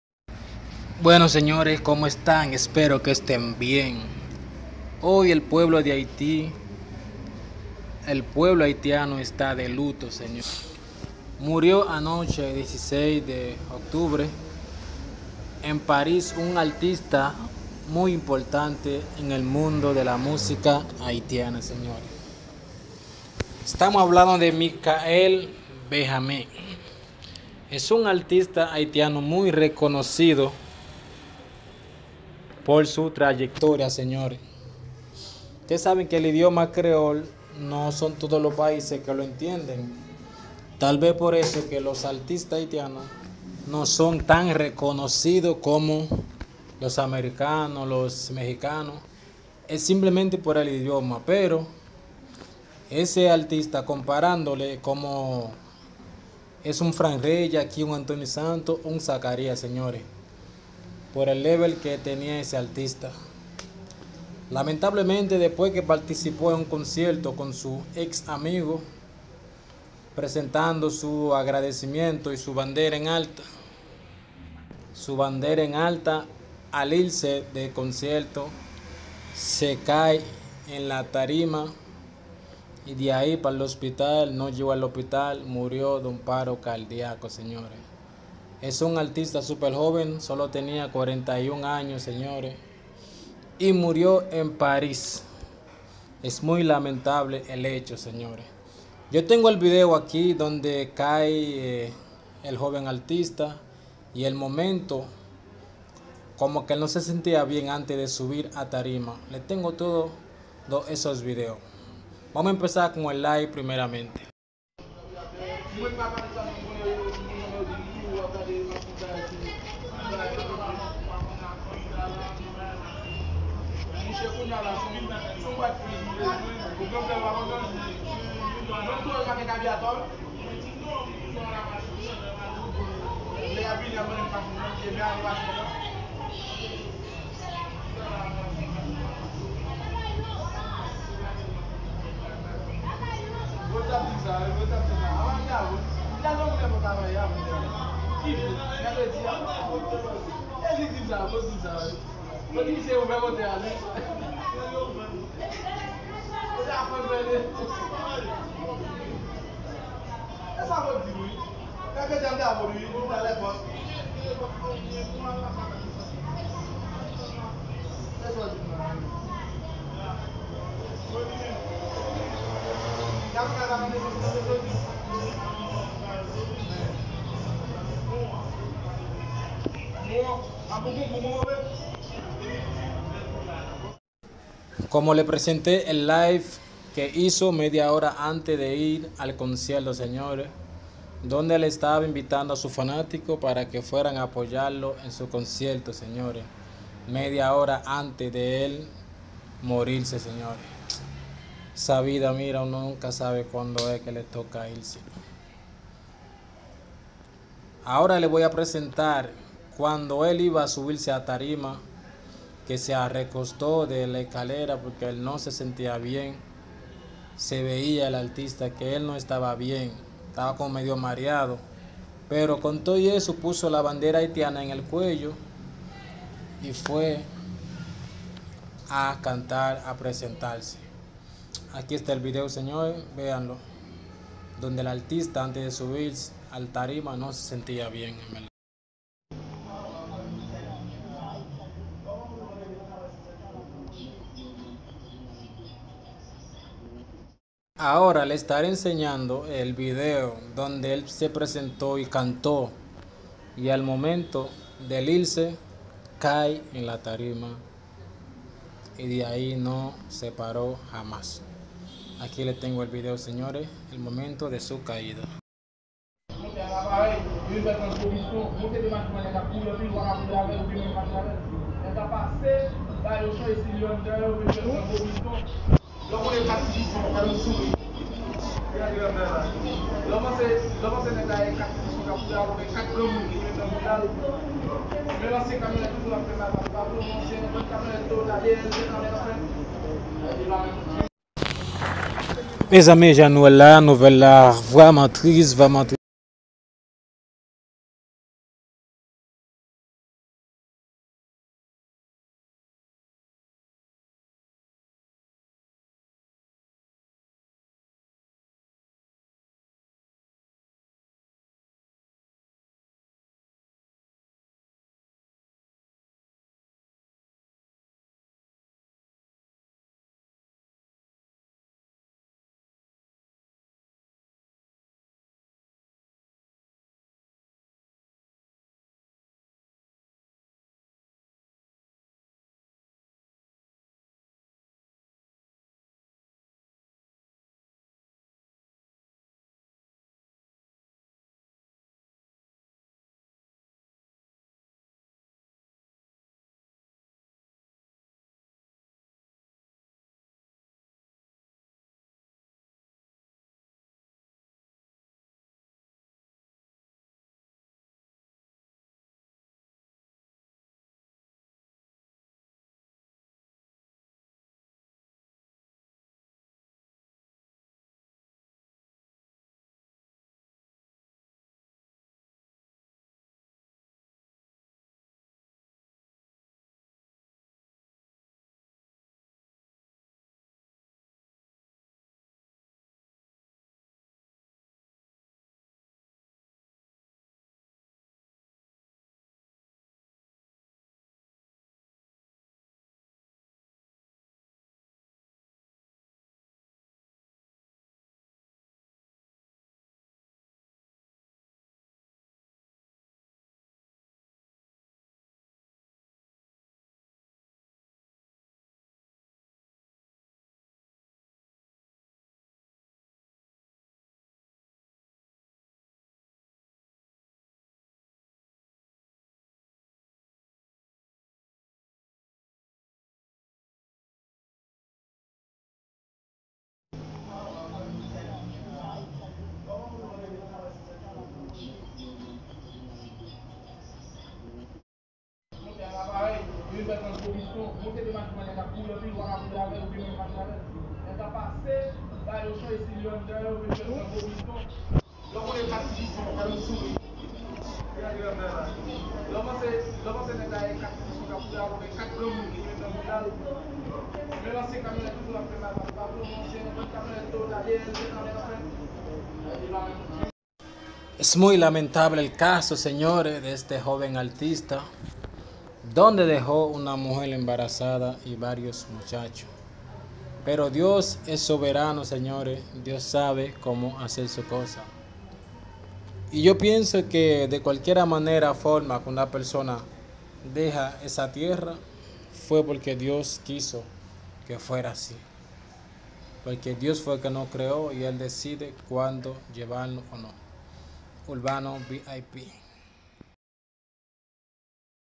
muere artista haitiano michael benjamín, muy reconocido, mientra estaba en un concierto en paris😭😭🗼
Rip mika el bejamin